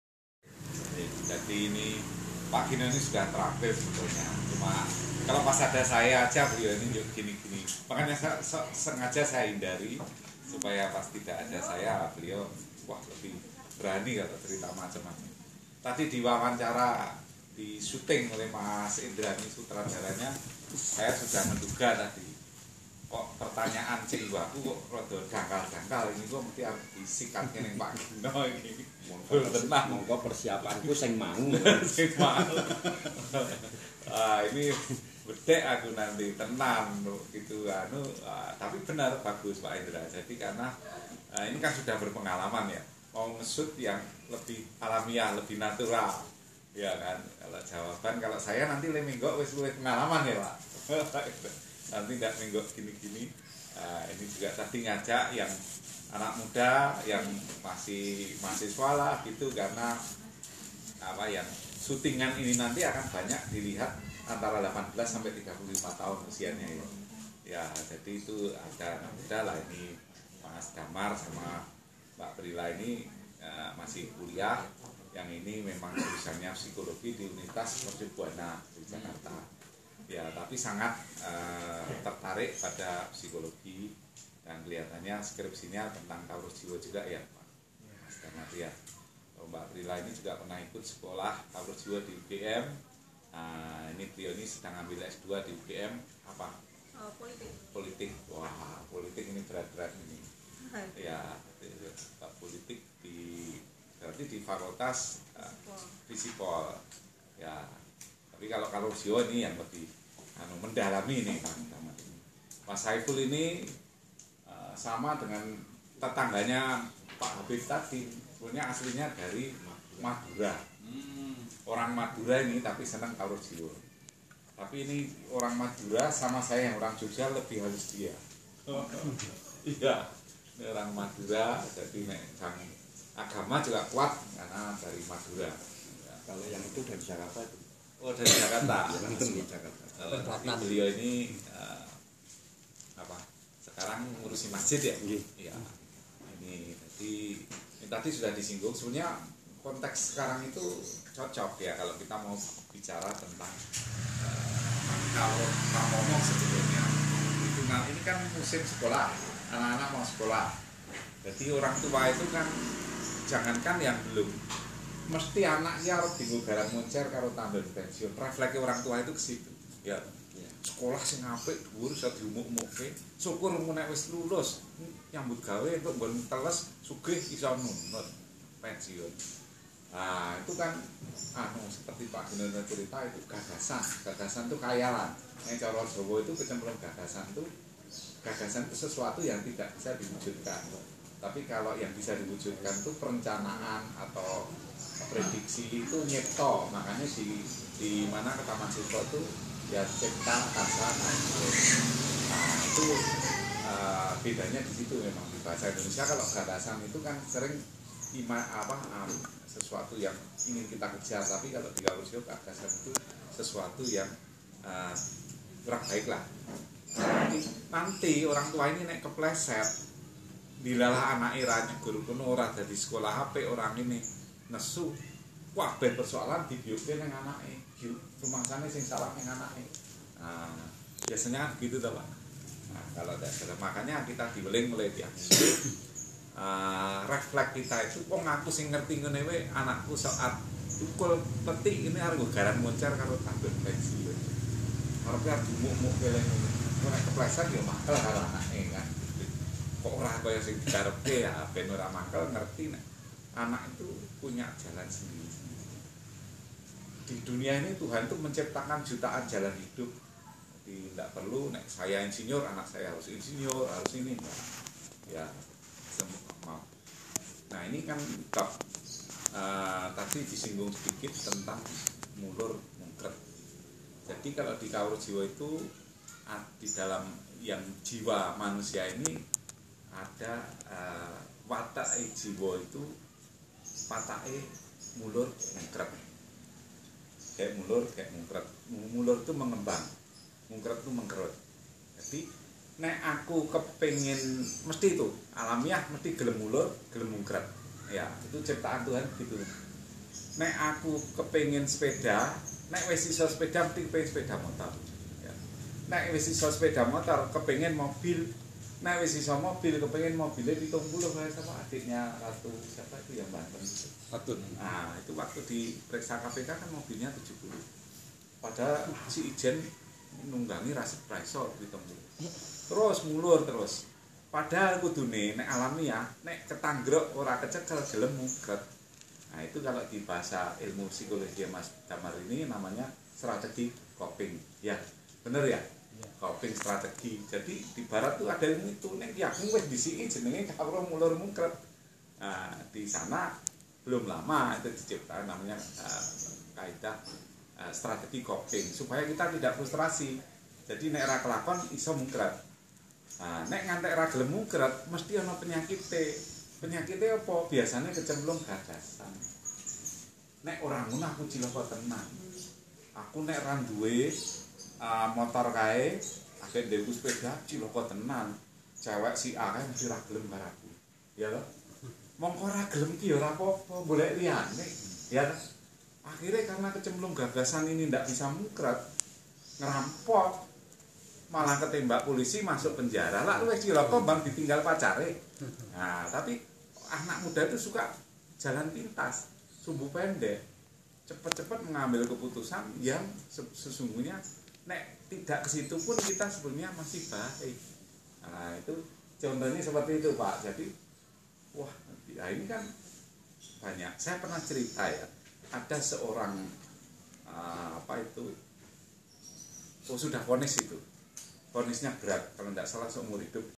Junggringan Ki Ageng Suryomentaram di dusun Balong, Yogyakarta Part.3
Junggringan Ki Ageng Suryomentaram di dusun Balong, desa Panggungharjo, Kecamatan Sewon, Kabupaten Bantul, D.I. Yogyakarta pada tanggal 25 Mei 2017. Junggringan dihadiri oleh Ki Gino, Ki Prasetyo, dan pelajar Kawruh Jiwo dari Yogyakarta dan Surabaya.